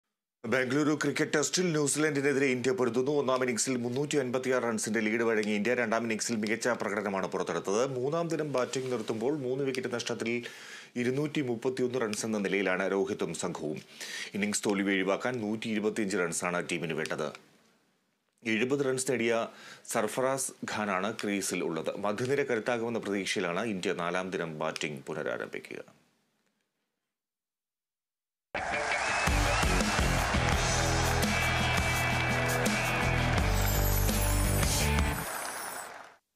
ബംഗളുരു ക്രിക്കറ്റ് ടെസ്റ്റില്‍ ന്യൂസിലാന്‍ഡിനെതിരെ ഇന്ത്യ
ബംഗളുരു ക്രിക്കറ്റ് ടെസ്റ്റില്‍ ന്യൂസിലാന്‍ഡിനെതിരെ ഇന്ത്യ പൊരുതുന്നു. ഒന്നാം ഇന്നിംങ്സില്‍ 356 റണ്‍സിന്റെ ലീഡ് വഴങ്ങിയ ഇന്ത്യ രണ്ടാം ഇന്നിങ്സില്‍ മികച്ച പ്രകടനമാണ് പുറത്തെടുത്തത്.  മൂന്നാംദിനം ബാറ്റിങ്ങ് നിര്‍ത്തുമ്പോള്‍ മൂന്ന് വിക്കറ്റ് നഷ്ടത്തില്‍ 231 റണ്‍സെന്ന നിലയിലാണ് രോഹിത്തും സംഘവും. ഇന്നിംഗ്‌സ് തോല്‍വി ഒഴിവാക്കാന്‍  125 റണ്‍സാണ് ടീമിന് വേണ്ടത്.  70 റണ്‍സ് നേടിയ സര്‍ഫറാസ് ഖാനാണ് ക്രീസിലുള്ളത്. മധ്യനിര കരുത്താകുമെന്ന പ്രതീക്ഷയിലാണ് ഇന്ത്യ നാലാം ദിനം ബാറ്റിങ്ങ് പുനരാരംഭിക്കുക.

About COA